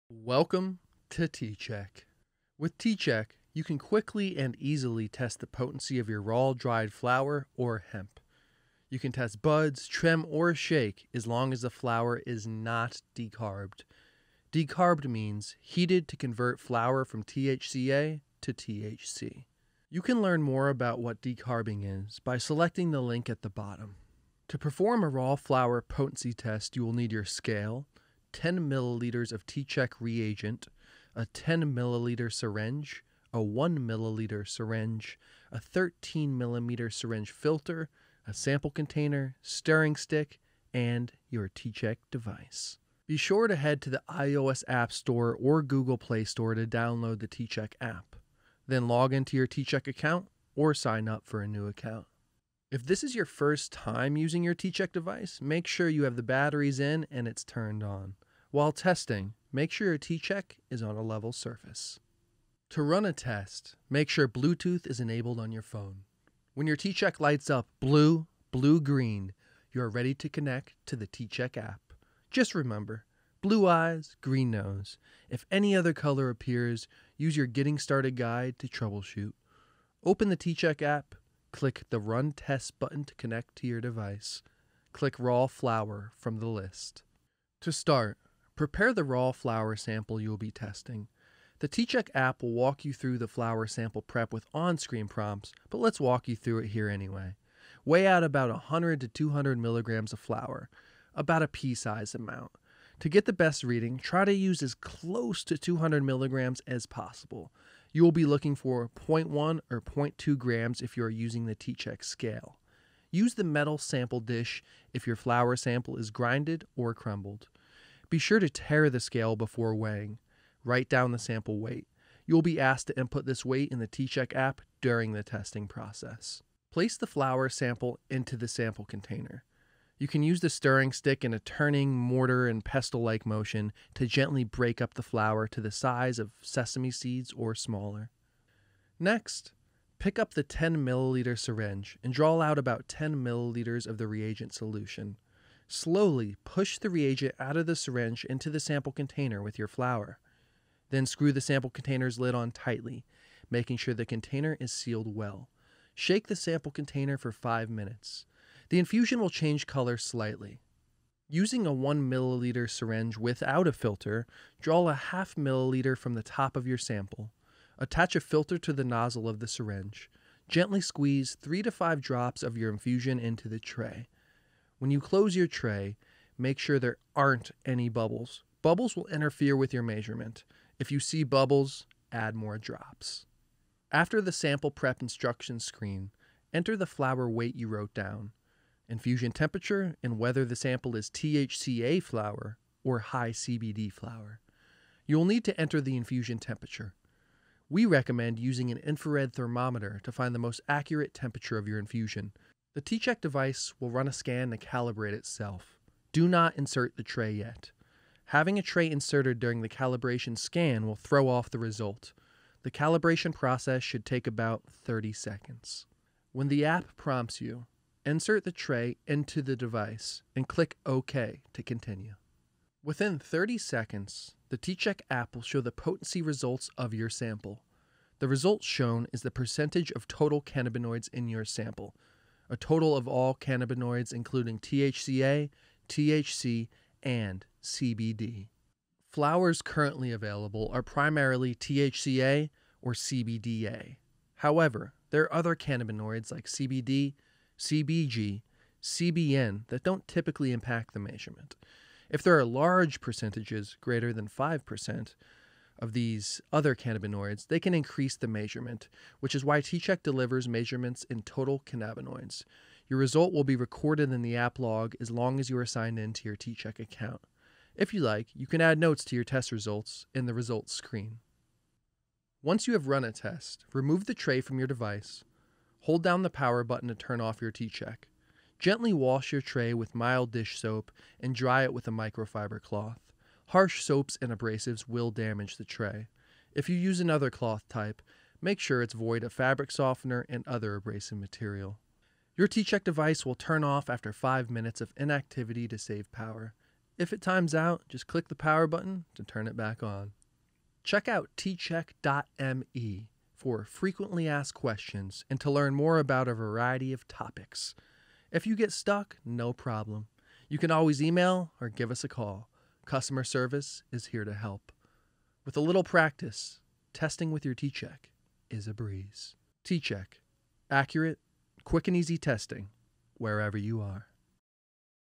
tCheck THC and CBD Potency Tester: How to Test Raw Flower
See our new tutorial video for using the tCheck THC & CBD Potency Tester to test raw flower infusions.